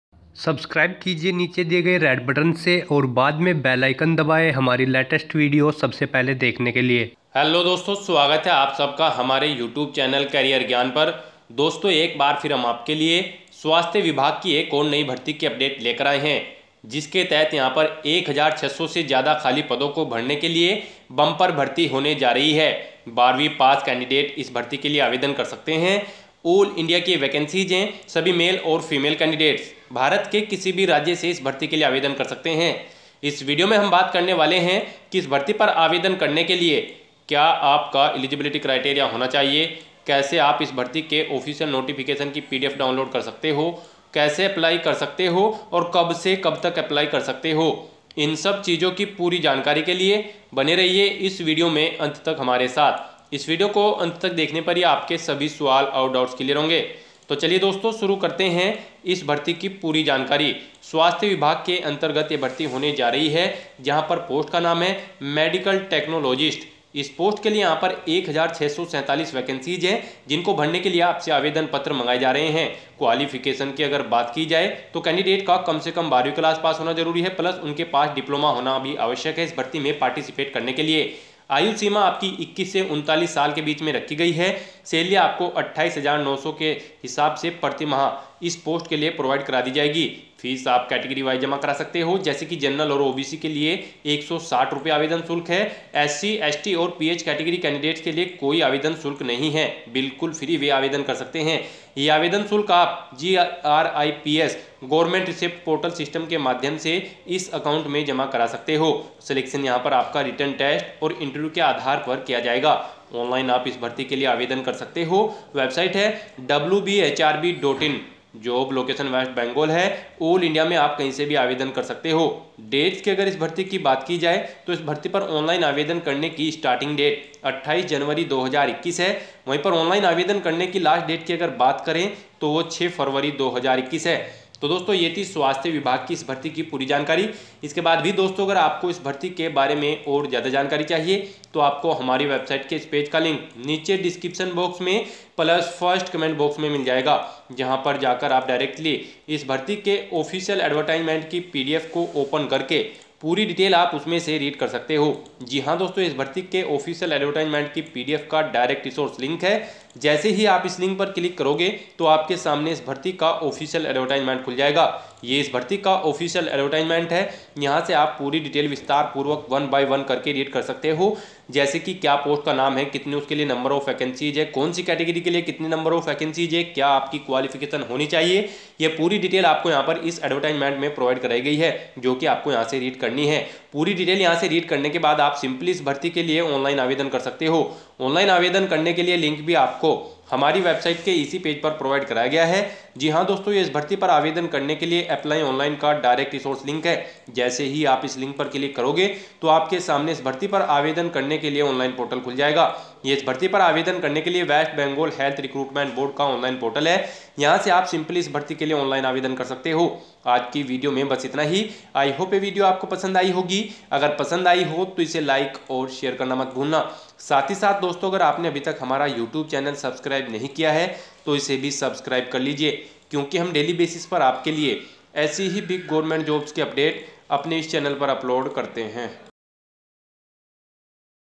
1647 पदों पर स्वास्थ्य विभाग में फिर निकली बड़ी भर्ती (Health Department Jobs 2021)
Hey guys, here in this video we are providing you details about Health Department Recruitment latest update. Recently application forms are inviting from Indian national eligible candidates by Health Department for fill up the post of Medical Technologist. Through this recruitment notification there are Large Numbers of vacancies will be fulfilled. Candidates who are eligible and liable for this recruitment opportunity, they can apply on or before last date. For more information related to this recruitment opportunity like eligibility criteria, selection process, application fee details, so please see this video till the end.